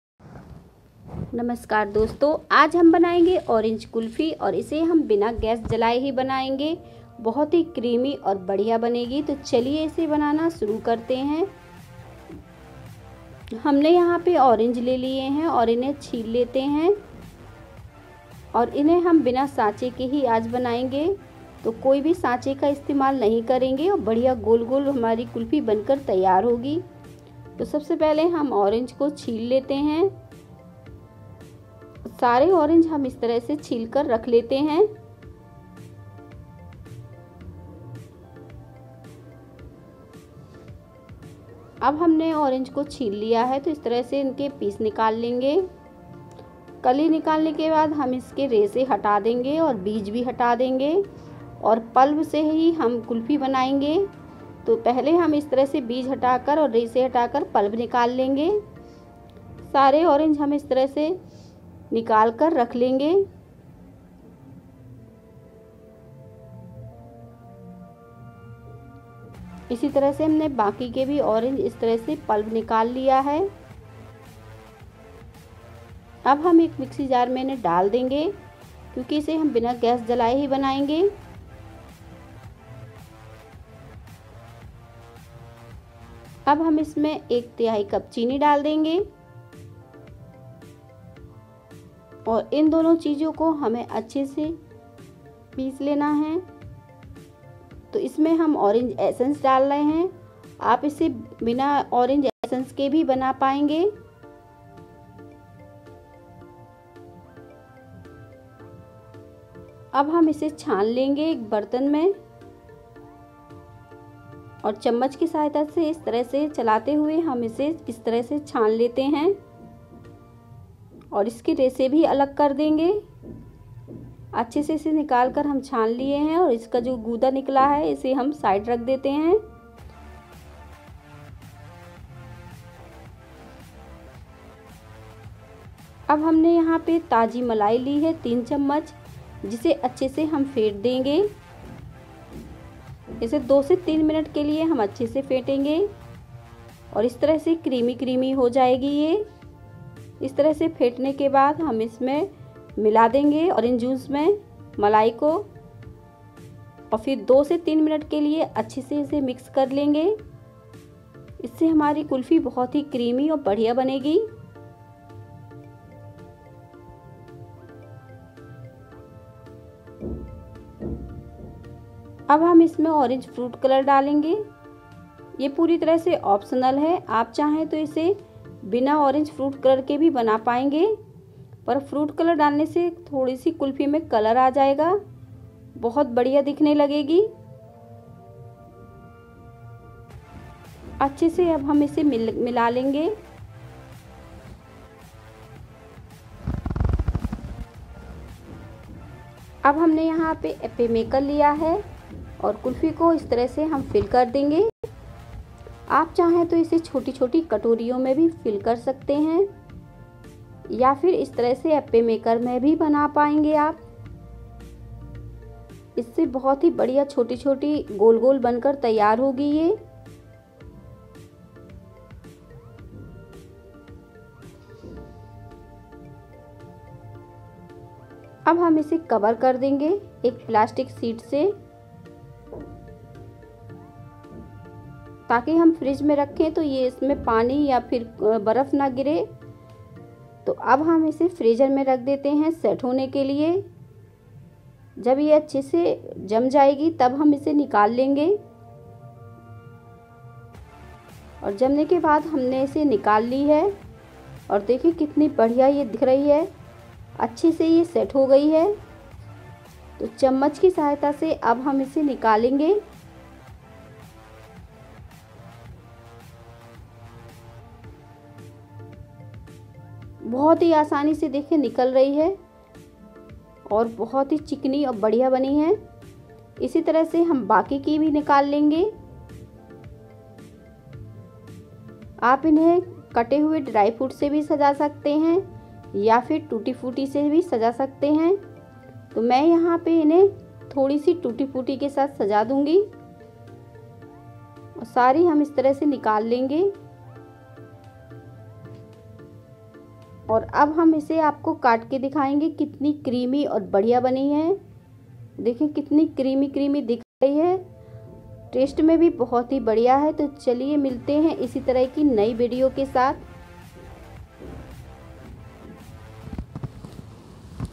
सिर्फ 3 चीजों से बनाए बिना गैस जलाए बिना आइसक्रीम मोल्ड के ओरेंज आइसक्रीम Orange icecream recipe
Ingredients-

Orange - 5-6
Sugar -1/3 cup
Orange essence-1 tsp (optional)
Orange colour-3-4 drops (optional )